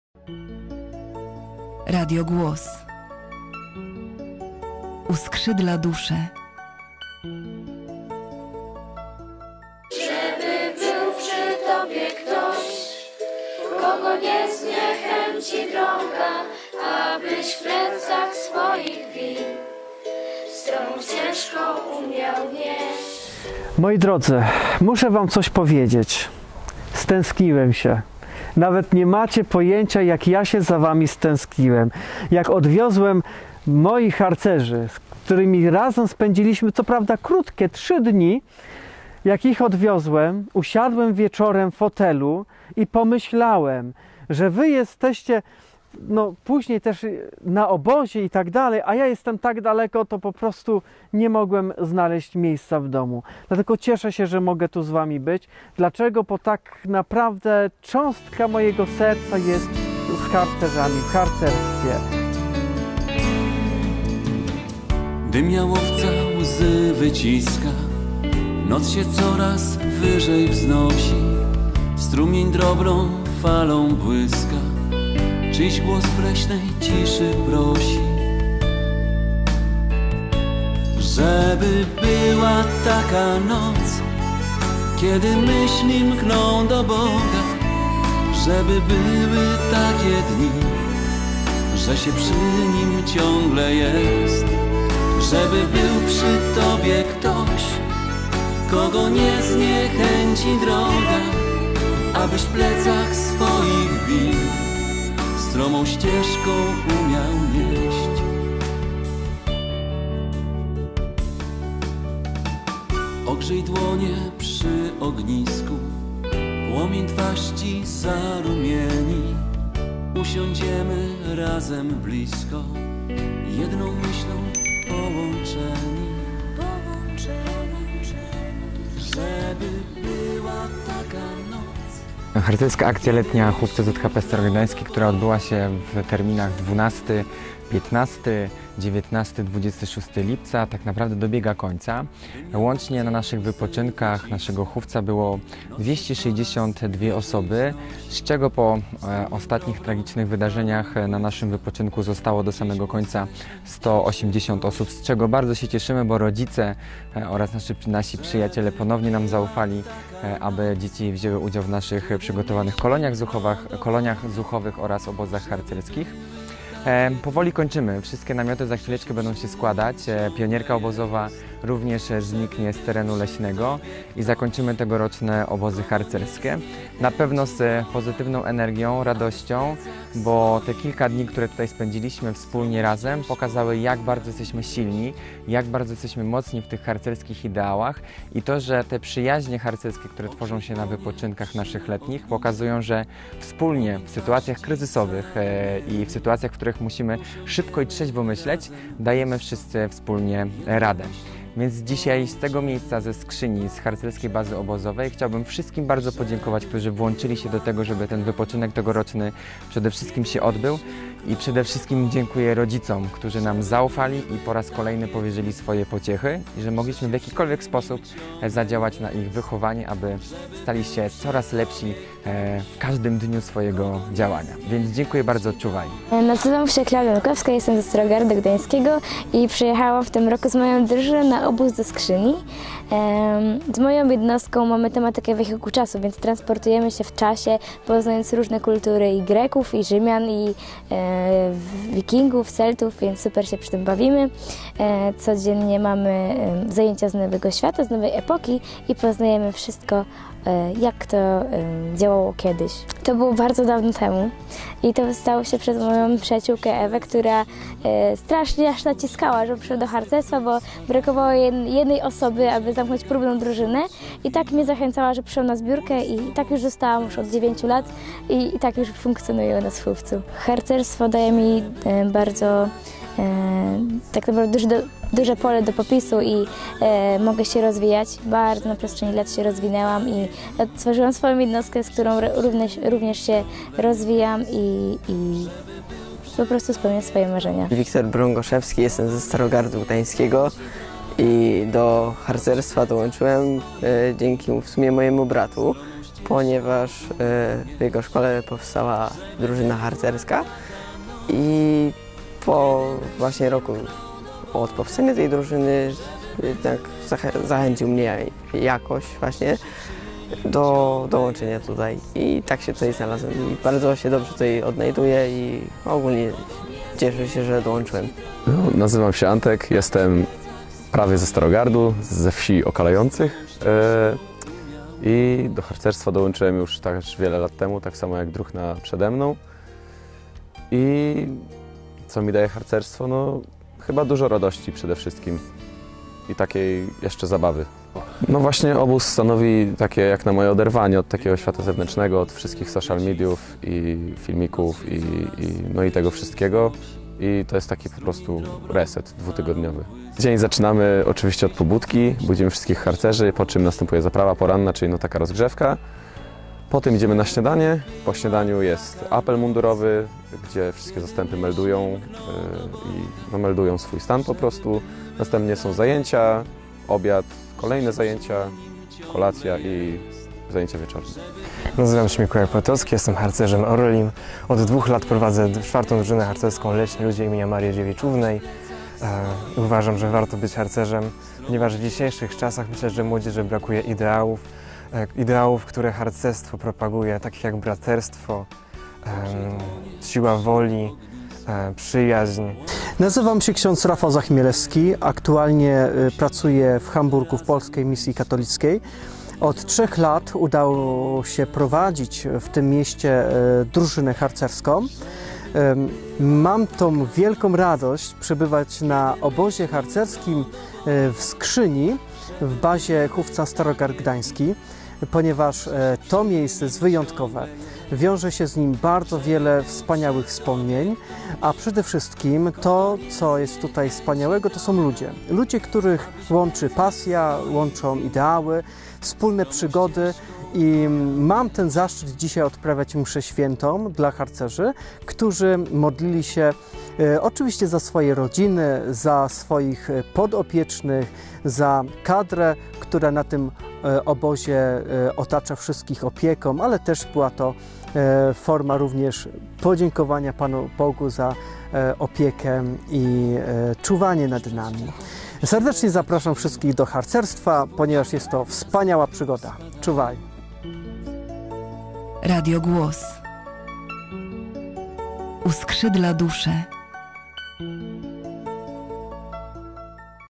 Harcerska akcja letnia hufca ZHP Starogard Gd. [reportaż]
Harcerska akcja letnia hufca ZHP Starogard Gdański w Skrzyni.
Reportaż